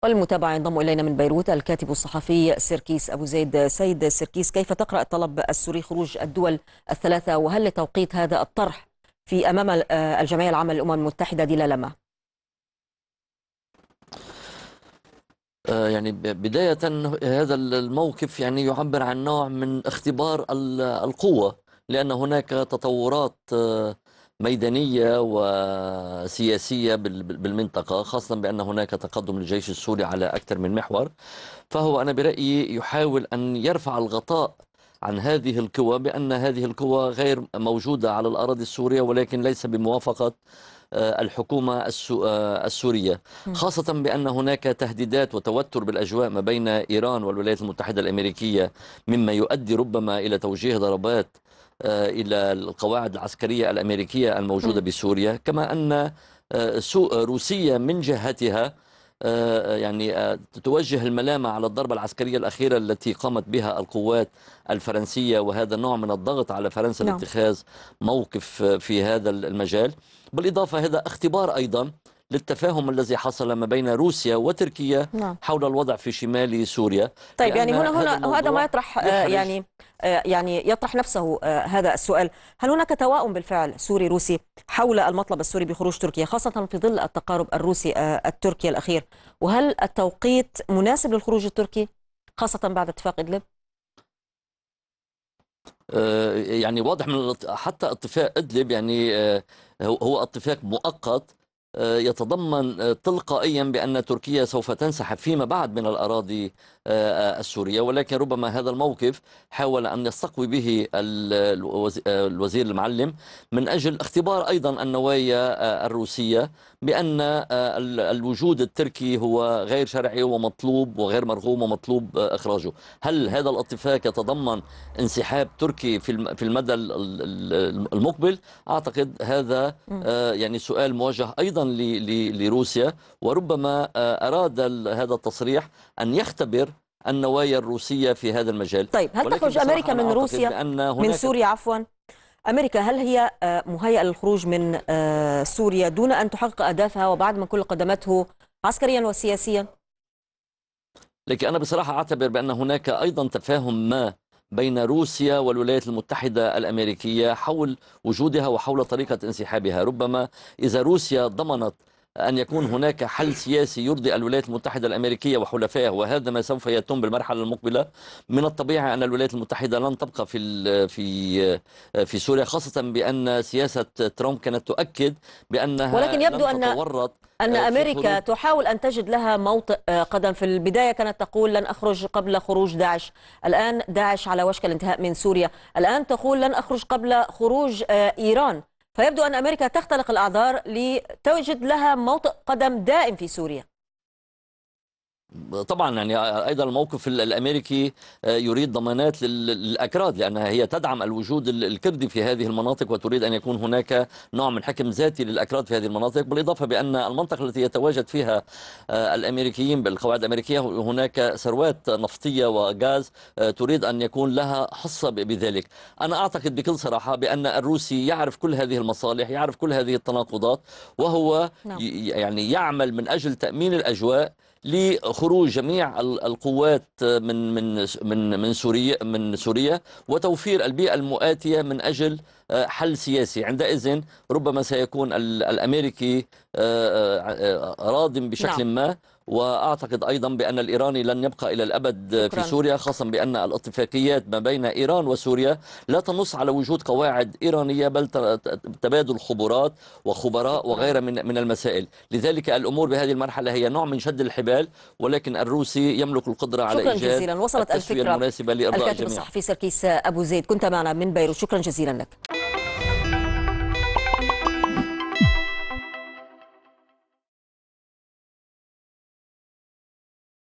صحفي: أمريكا تريد ضمانات للأكراد قبل خروجها من سوريا
اتهم وزير الخارجية السوري، وليد المعلم، التحالف الدولي بقيادة الولايات المتحدة، بالعمل على نشر الفوضى والإرهاب في سوريا، تماشيا مع أهداف المجموعات الإرهابية، التي يقدم لها الدعم العسكري المباشر، وفي كلمته أمام الجمعية العامة للأمم المتحدة، طالب المعلم القوات الأمريكية والفرنسية والتركية بالانسحاب فورا من سوريا، مؤكدا أن معركة سوريا ضد الإرهاب شارفت على الانتهاء.. مزيد من التفاصيل يتحدث سركيس أبو زيد الكاتب الصحفي مع الإعلامية هبة الغمراوي.